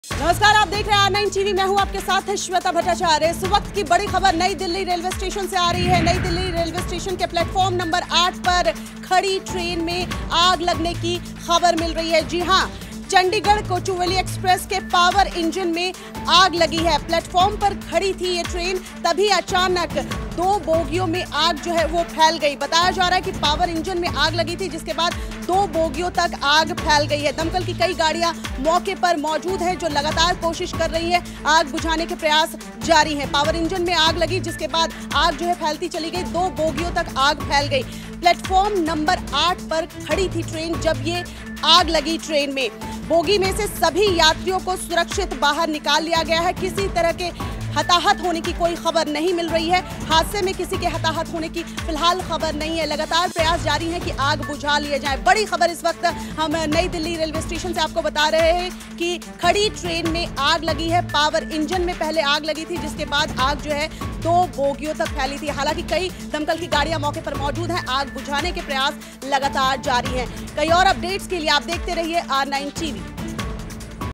NEW DELHI RAILWAY STATION पर बर्निंग ट्रेन
नई दिल्ली रेलवे स्टेशन के प्लेटफॉर्म 8 पर खड़ी चंडीगढ़-कोचुवेली के पावर इंजन में लगी आग... फायर ब्रिगेड की कई गाड़ियां मौके पर मौजूद